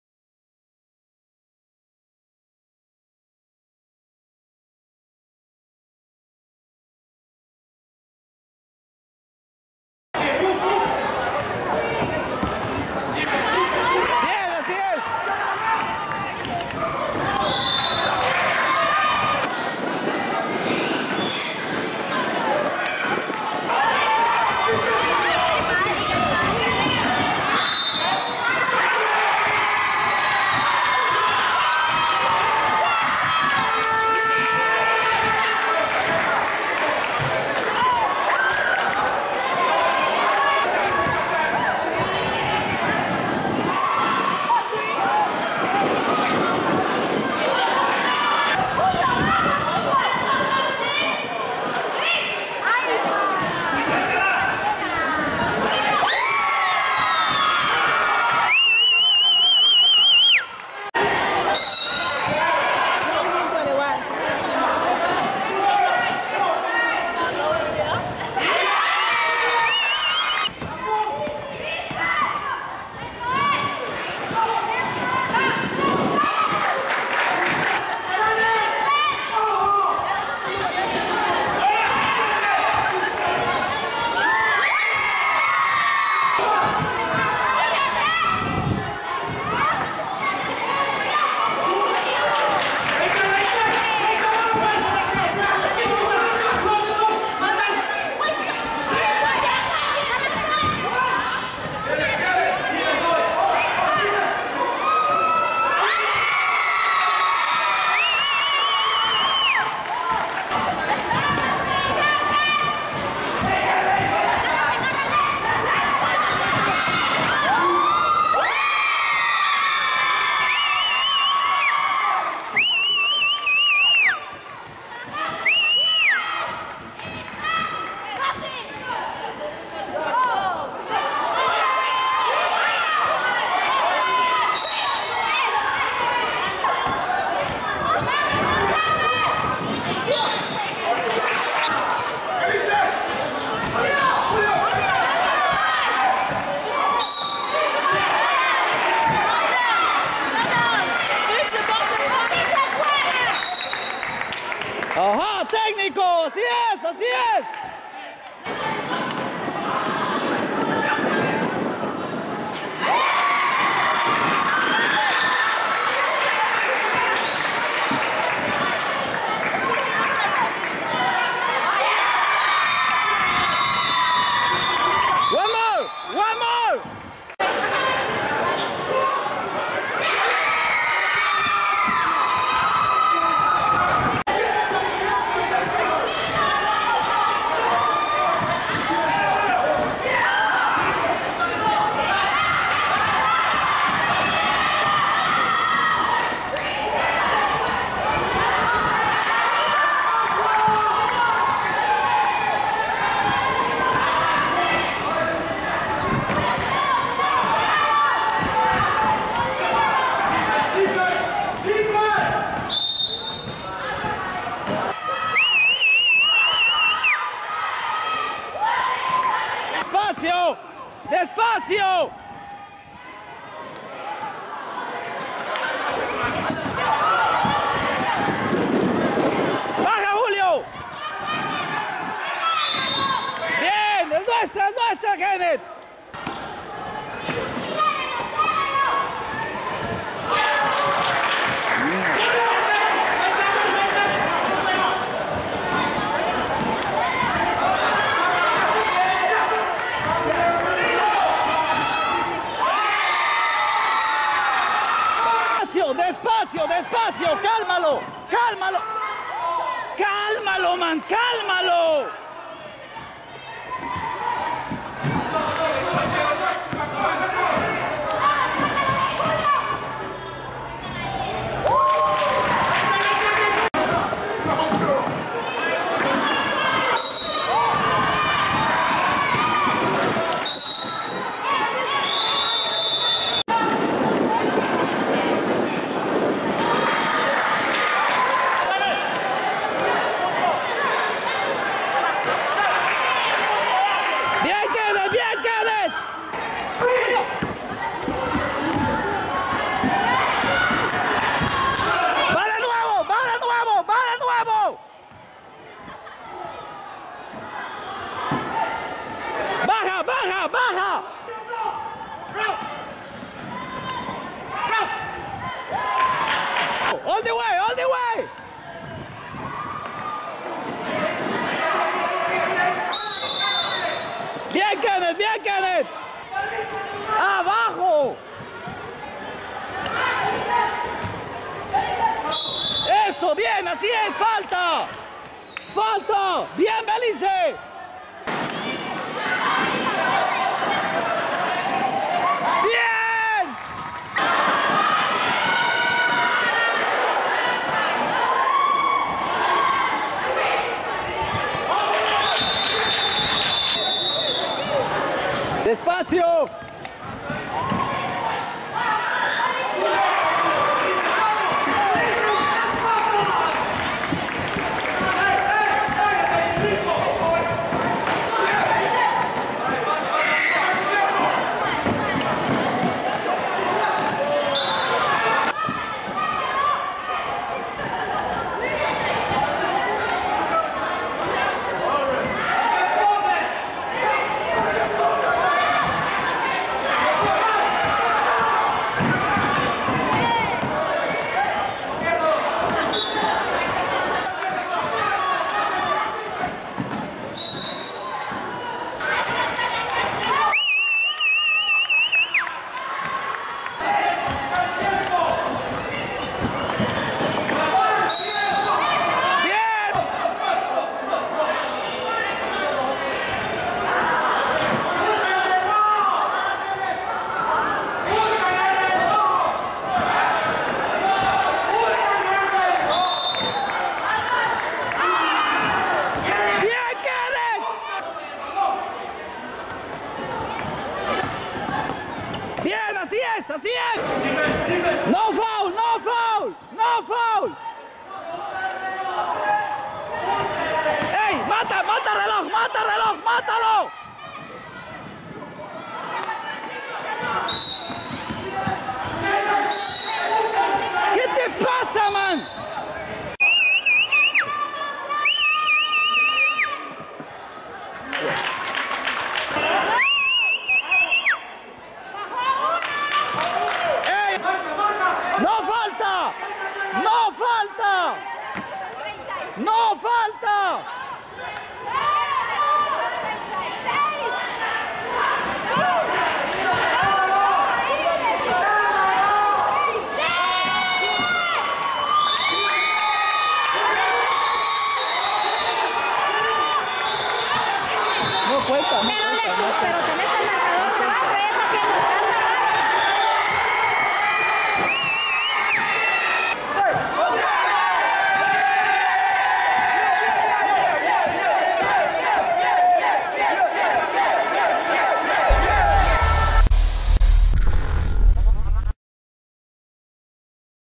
NATIONAL CHAMPIONSHIP FINALS: Mount Carmel vrs San Pedro_p2
Mount Carmel CHAMPS ! !
Honorable Mention: Kenneth Aguirre
Belize, feb 06 2015 . . .